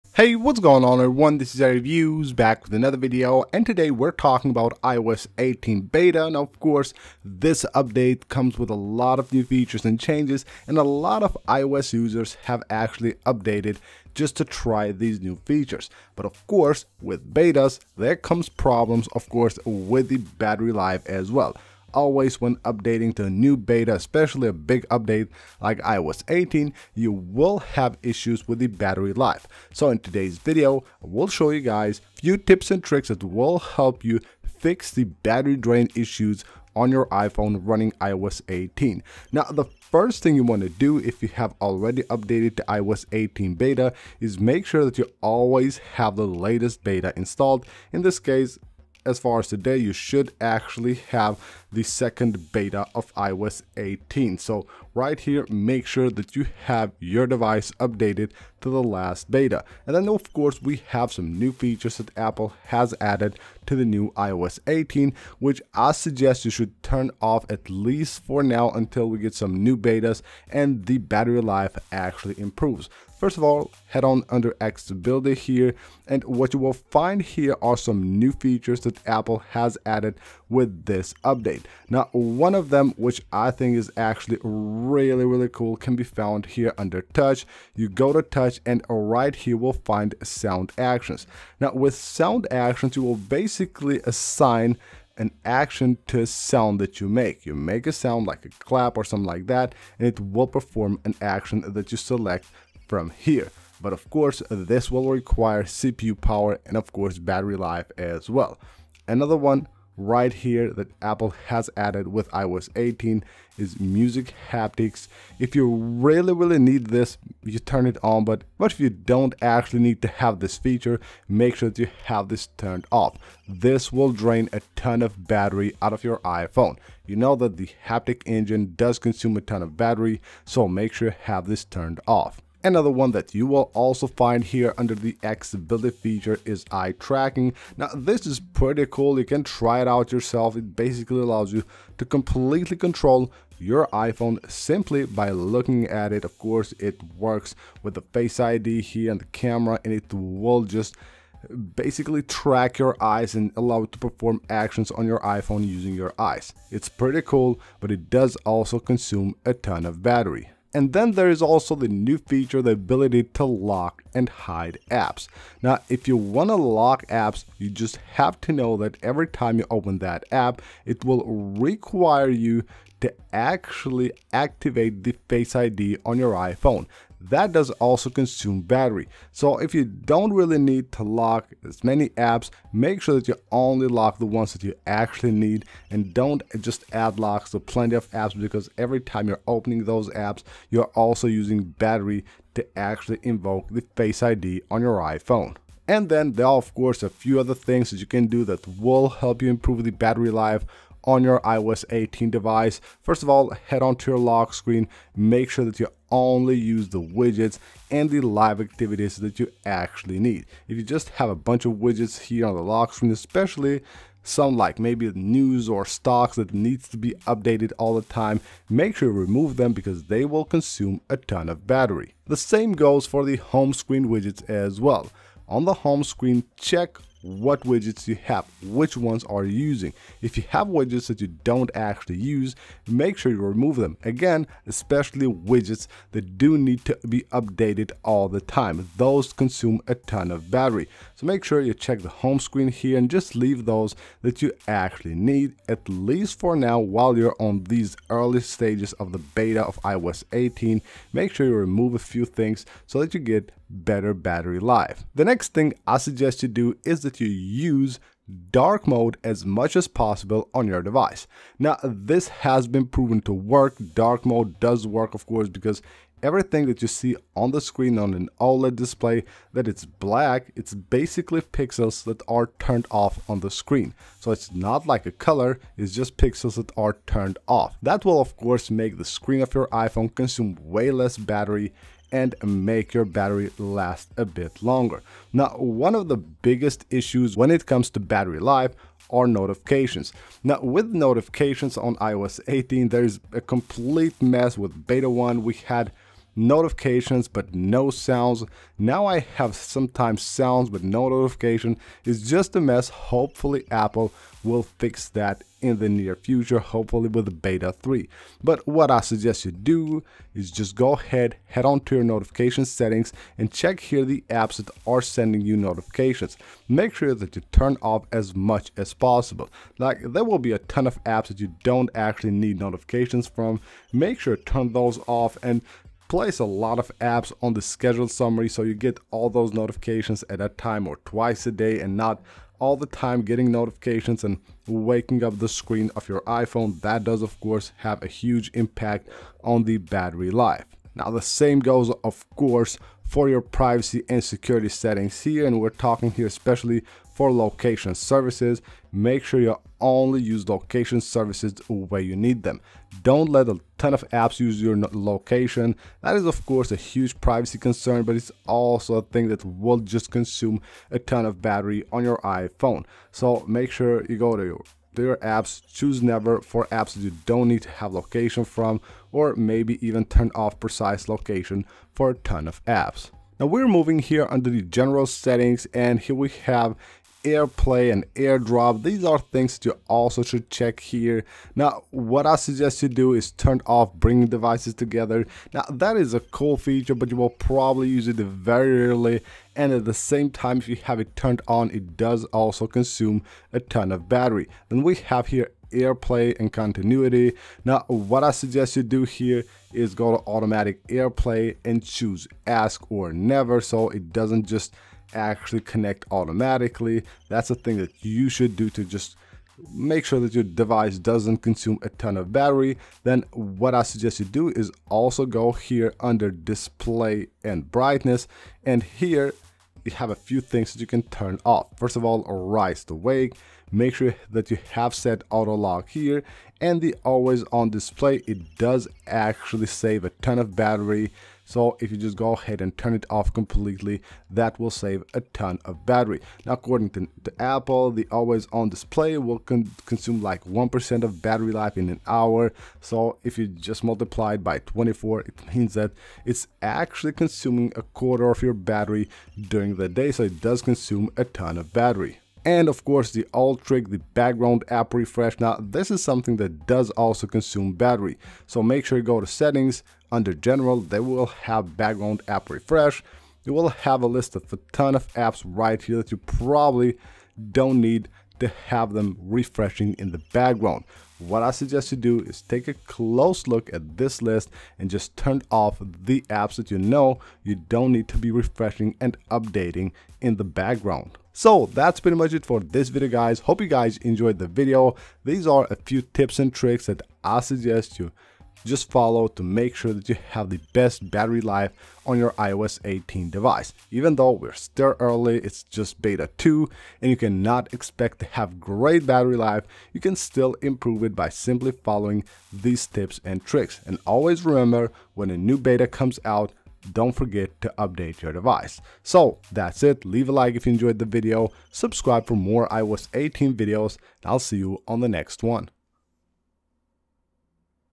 iOS 18 Beta - How To FIX Battery DRAIN on iPhone!
iOS 18 beta has recently been released with new features and changes. iOS 18 beta brings new features and changes but you might not get the best battery life. So in this video I will show you how to fix battery drain on iOS 18.

New on iOS 18 0:00
Lock Screen 3:15
Home Screen 3:42
Important Settings 4:37